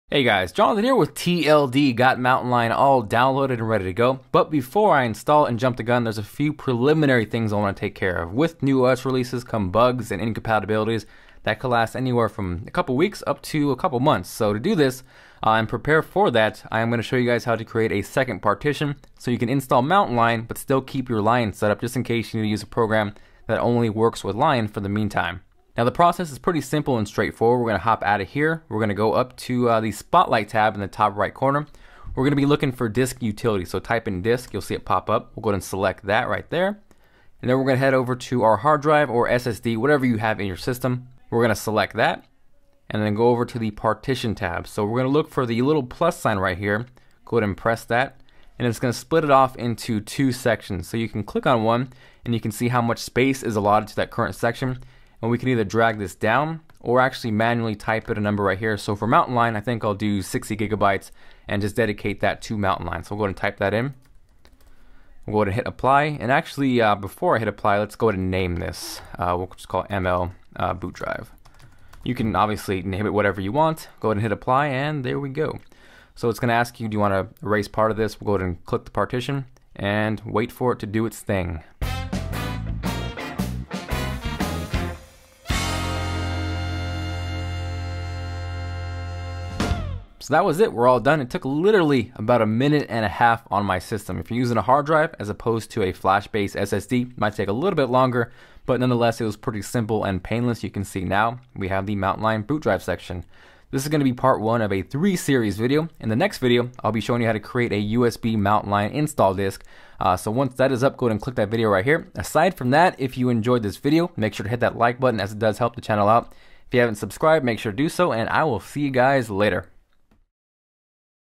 How to Create a 2nd Partition for a Dual Boot Mountain Lion 10.8 / 10.7 Lion Setup
You can hit me up on Twitter for any questions!

With Mountain Lion 10.8 hitting the App Store today, there are tons of people quick to jump the gun and install right away but I usually like to wait a few weeks to months whenever a new OS is released before I make it my "Daily Driver. In this video I show you how you can created a 2nd partition to install Mountain Lion 10.8 but still keep your original 10.7 Lion installed for a dual-boot setup.